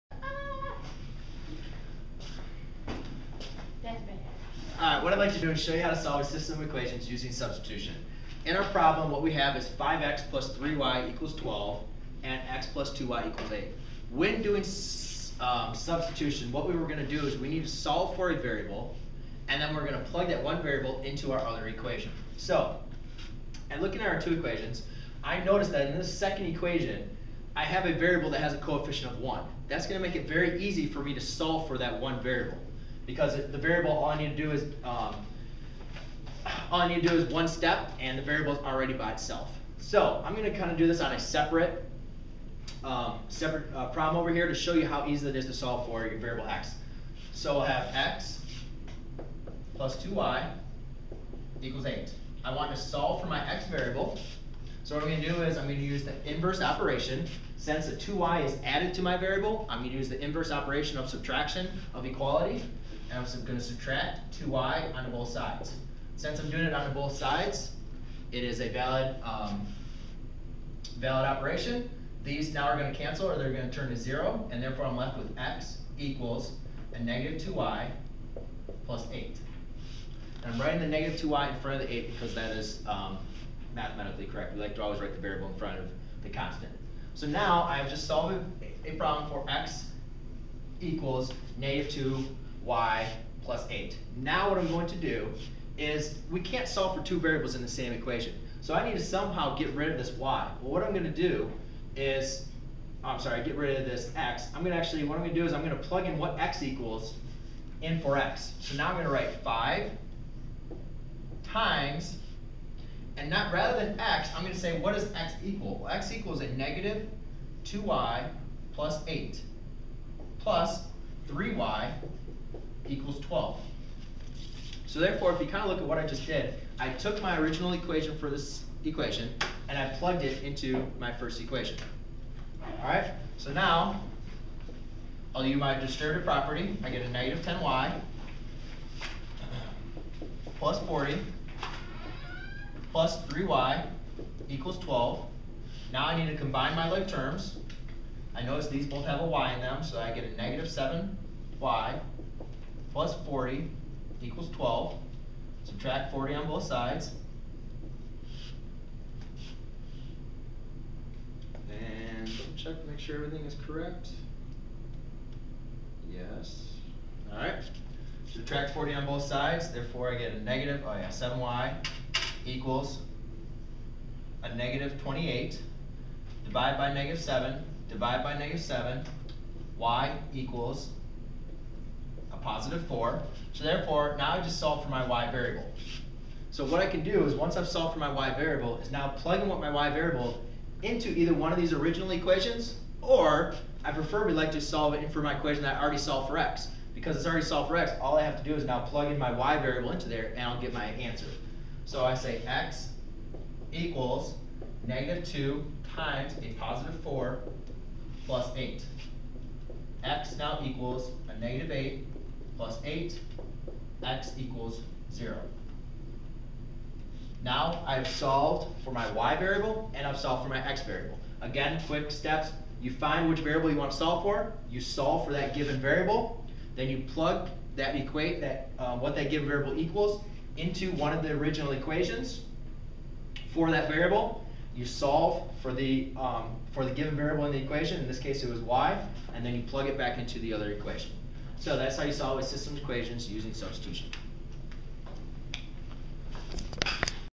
How to solve a system of equation using substitution
👉Learn how to solve a system of equations by substitution. To solve a system of equations means to obtain a common values of the variables that makes the each of the equation in the system true. To solve a system of equations by substitution, we solve for one of the variables in one of the equations and then substitute for the variable in the other equation and then solve for the other variable. Next we plug in the obtained value of the other variable into the equation for the initial variable we solved for to obtain the value of the variable.

The value of the two variables obtained is the solution to the system of equations.